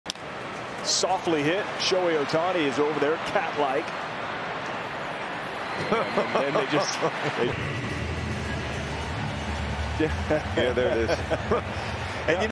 C’mon, we both know how this ends. (Shohei Ohtani is too kind). 🤣
Shohei Ohtani politely tags Kyle Tucker on a ground all!